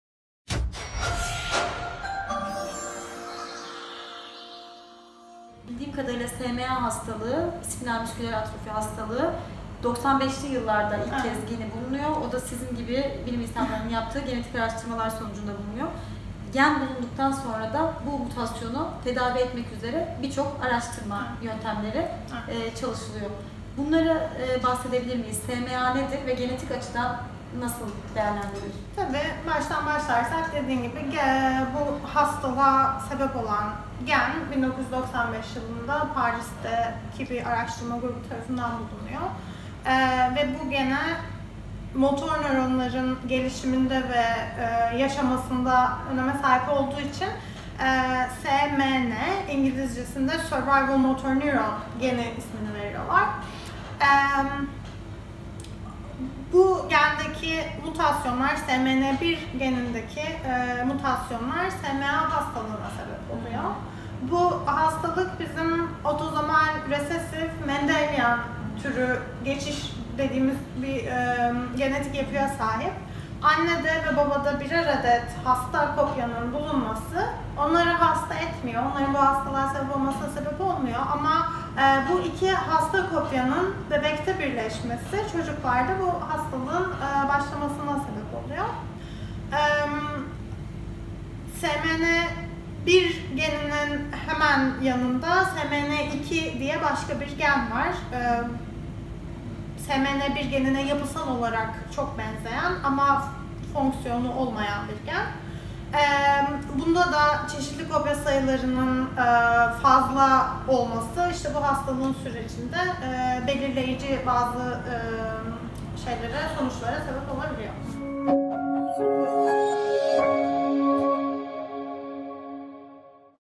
SMA Farkındalık Ayı Bilgilendirici Video Serisi - 2
🦋 SMA hastalığına yol açan gen, ilk kez 1995 yılında Paris'te bir grup araştırma görevlisi tarafından bulunmuştur. Gen bulunduktan sonra, bu mutasyonu tedavi etmek üzere birçok araştırma yöntemi geliştirilmiştir. Peki SMA nedir? Genetik açıdan nasıl değerlendirilir? SMA ayına özel hazırladığımız bilgilendirici video serimizin ikinci bölümü sizlerle 🌸